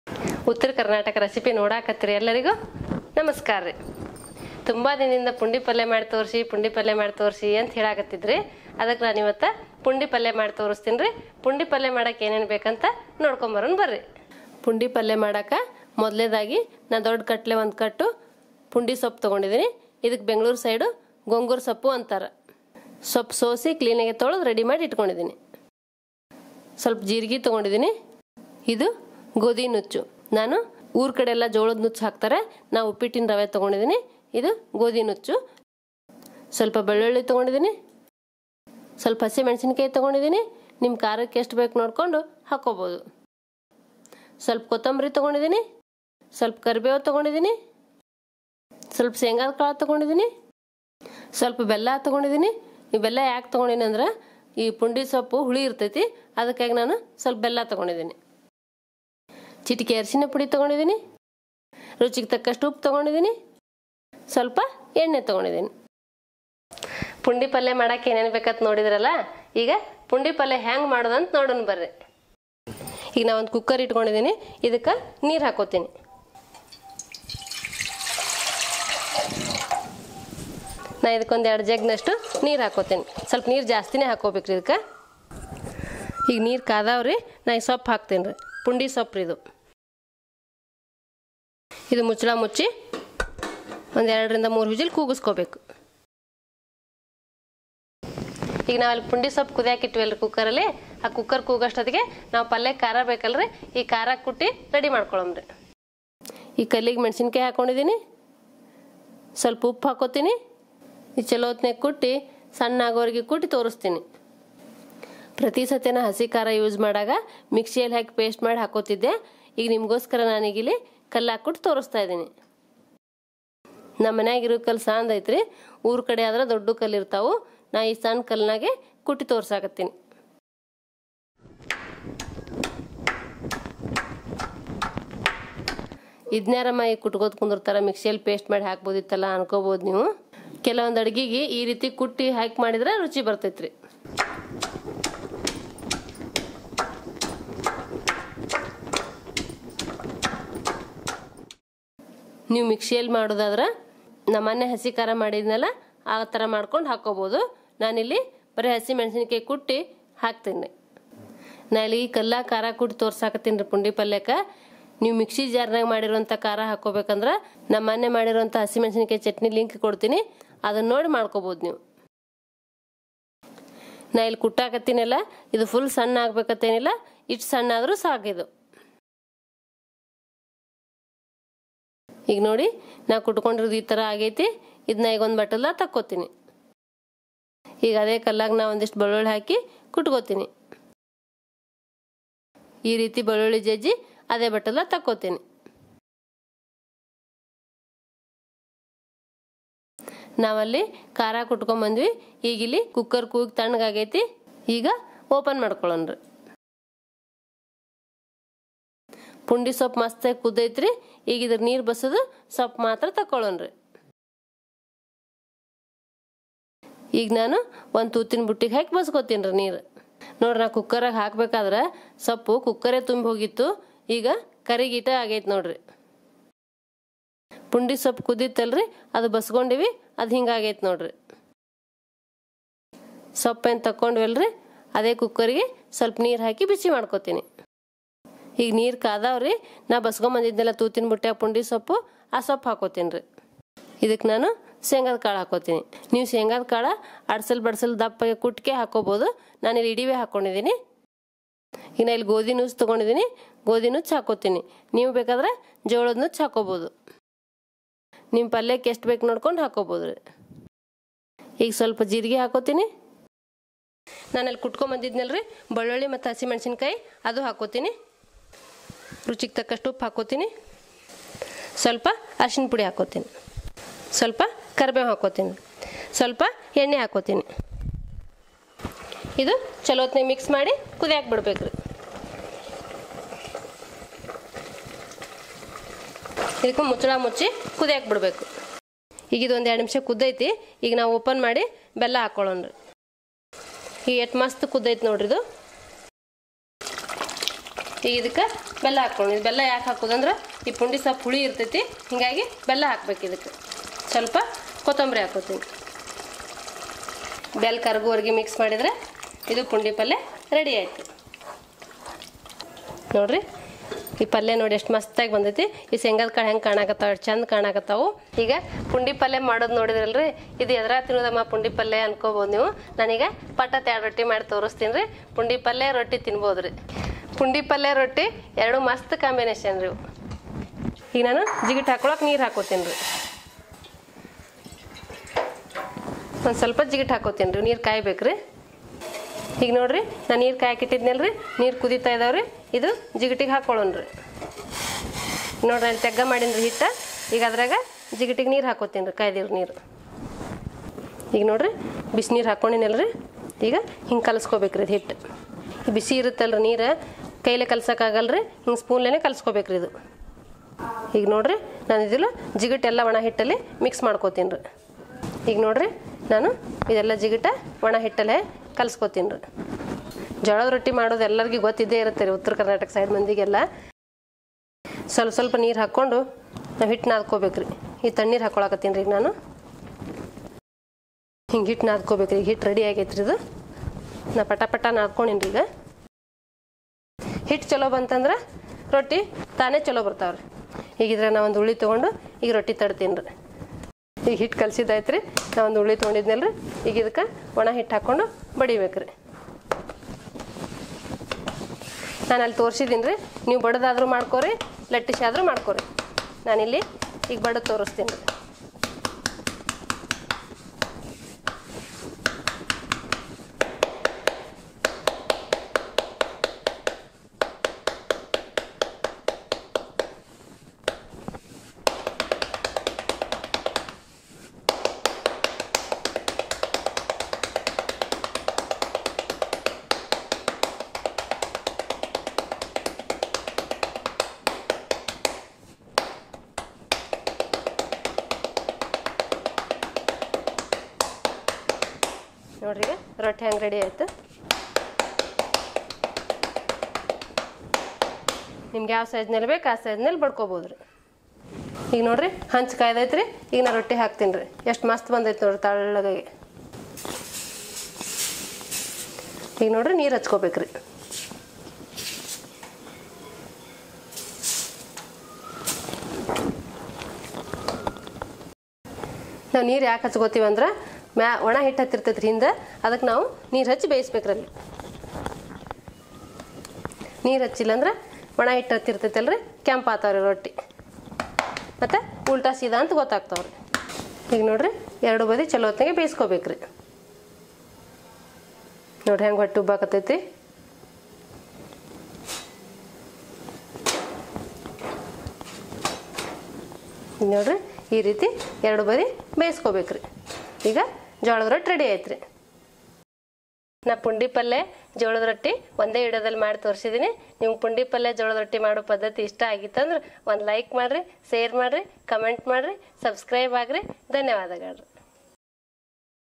Pundi Palya|Pundi Palya Recipe|Palya For Jolada Rotti||Gongura Soppu Palya|Uttara Karnataka Recipe
Pundi Palya is a palya serve with Jolada Rotti.  Pundi palya is very popular in North Karnataka. It is also known as Gongura soppu. Today i prepared pundi palya and Jolada Rotti in one video
 ...ನಮಸ್ಕಾರ ವೀಕ್ಷಕರೇ ಮೊನ್ನೆ ಜೋಳದ ರೊಟ್ಟಿ ಮಾಡಿದಾಗ ಪುಂಡಿ ಪಲ್ಯ ಮಾಡಿ ಅಂತ ತುಂಬಾ ಜನ ಕೇಳಿದ್ದೀರಿ ಲಾಕ್ ಡೌನ್ ಇರುವದರಿಂದ ನನಗೆ ಪುಂಡಿ ಸೊಪ್ಪು ಸಿಗುವುದು ಲೇಟ್ ಆಯಿತು ಕ್ಷಮೆ ಇರಲಿ ಈಗ ಪುಂಡಿ ಸೊಪ್ಪು ಸಿಕ್ಕಿದೆ ಹಾಗಾಗಿ ಜೋಳದ ರೊಟ್ಟಿ ಮತ್ತು ಪುಂಡಿ ಪಲ್ಯ ಒಂದೇ ವಿಡಿಯೋದಲ್ಲಿ ಮಾಡಿ ತೋರಿಸಿದ್ದೇನೆ

Ingredients  for pundi palya
pundi (gongura)soppu
cumin seeds
godhi nuchu
garlic
green chilli
coriander leaves
curry leaves
ground nut/shenga
jaggery
turmeric powder
salt
oil